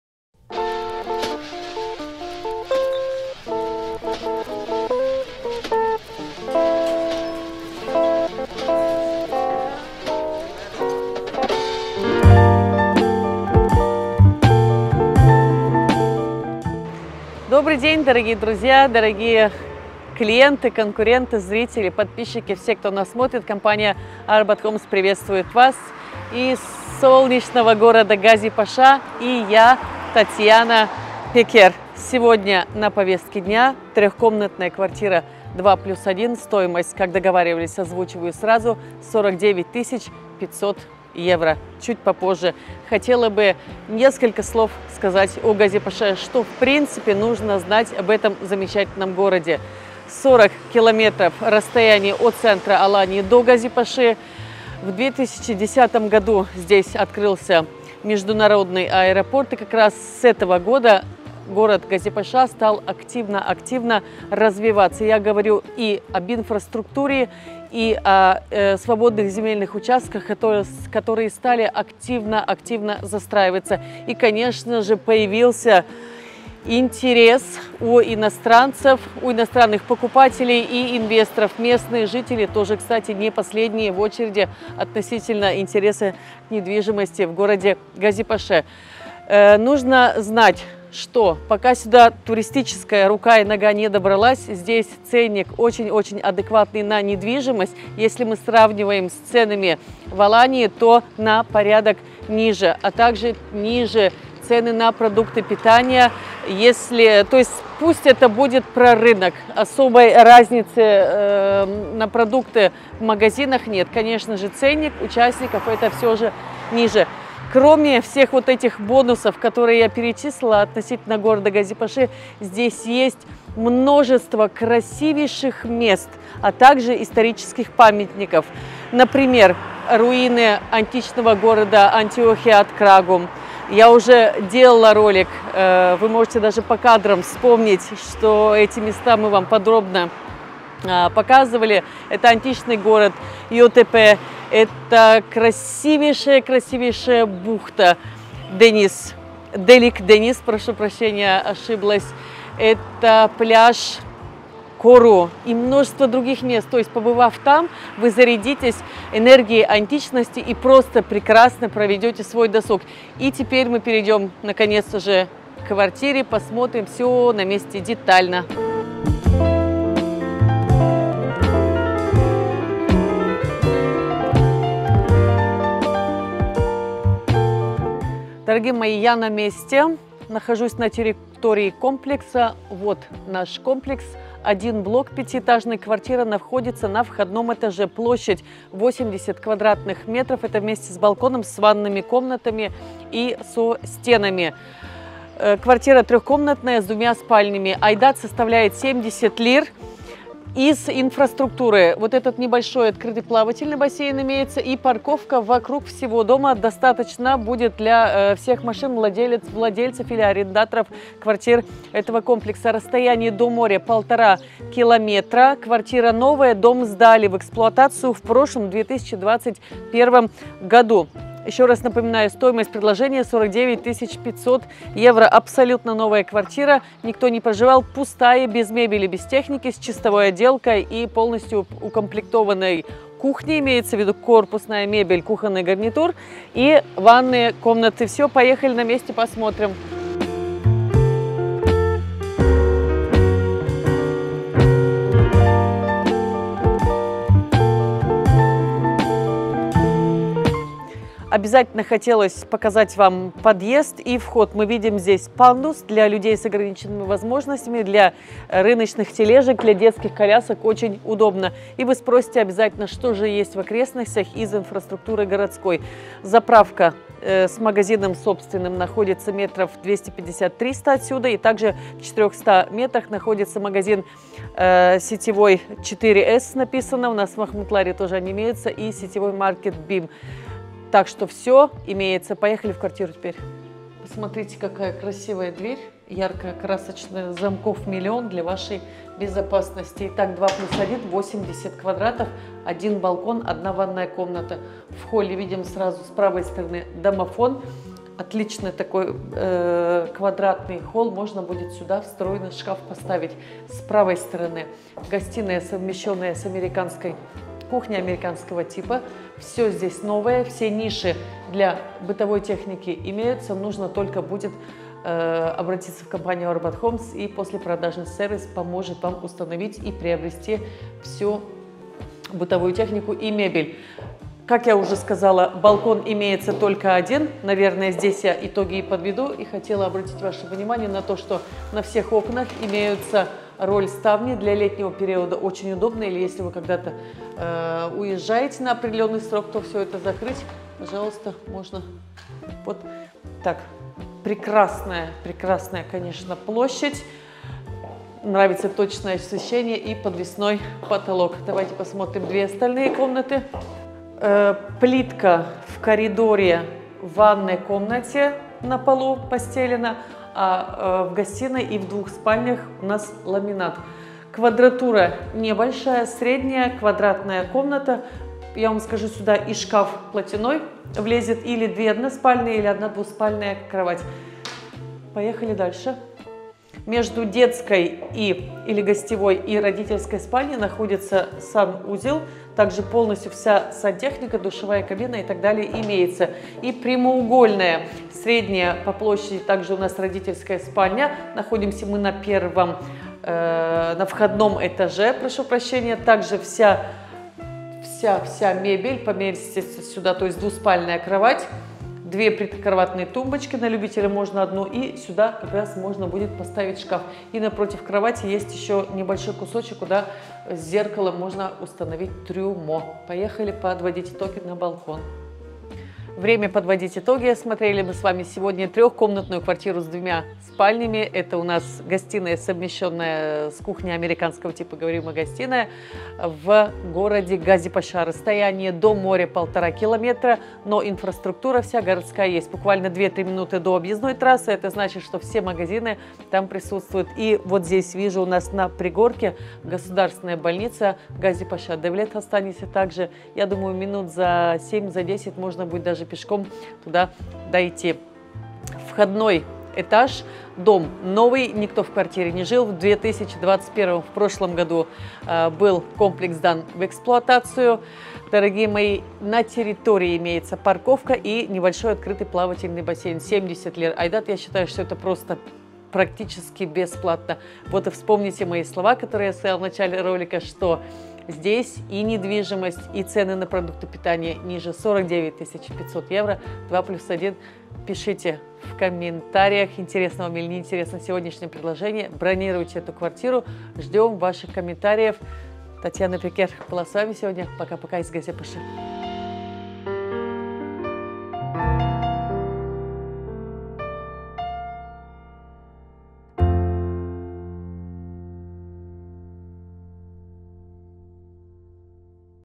Газипаша / Аланья / Недвижимость в Турции / Апартаменты в Алании / Турция 2022
00:00Недвижимость в Турции
03:23Инфраструктура возле дома
04:46 Обзор квартиры возле моря
06:56Итоги обзора

Центральную часть средиземноморского побережья Турции часто называют Турецкой Ривьерой, и именно тут находится прекрасная курортная Аланья. С одной стороны город расстилается на побережье Средиземного моря, с другой его окутывают горы 🏖 🌊⛰
Весь город покрыт зеленой растительностью, тут много аллей и зон отдыха🌴🌳🏝
Современные жилые комплексы имеют просто огромнейшие территории с бассейнами, садами с фруктовыми деревьями, беседками, детскими и спортивными площадками, парковками и охраной. И, конечно же, тут просто потрясающие виды из окна буквально из любого дома! 🏞🌅 Еще тут невероятно большие площади квартир и очень продуманные планировки, которые одновременно и удивляют, и впечатляют!🙂
Агентство недвижимости «ArbatHomes»  поможет вам с покупкой недвижимости в Турции!🇹🇷